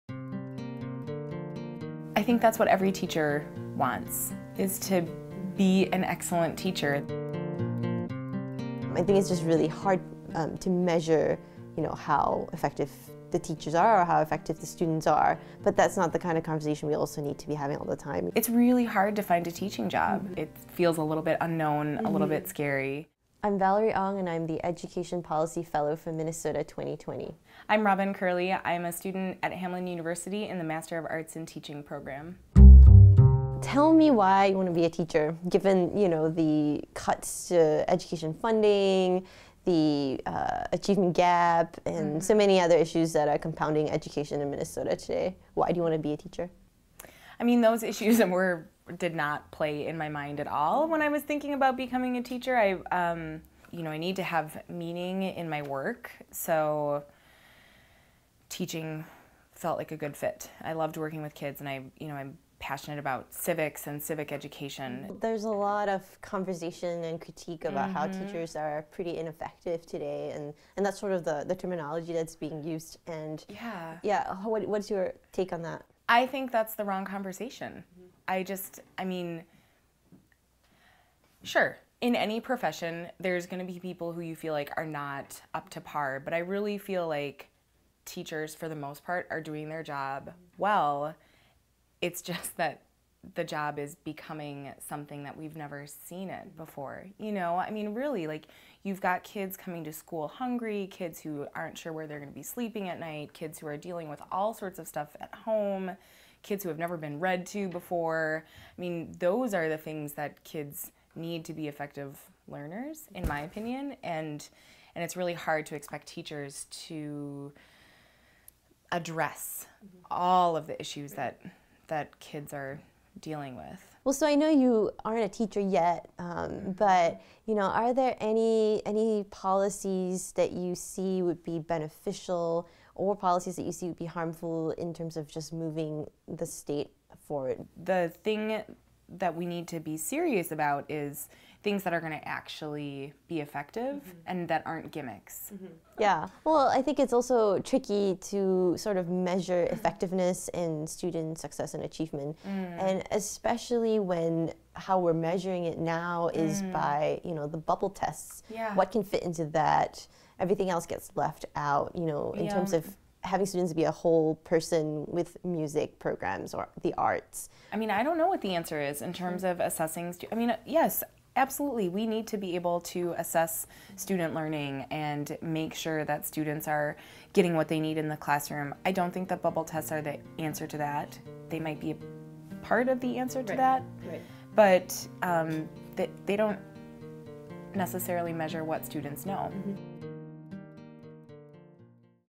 MN 2020: Kitchen Table Conversation with Educator
With the assault on the profession, why teach. Minnesota 2020 talks to one soon-to-be teacher to find out.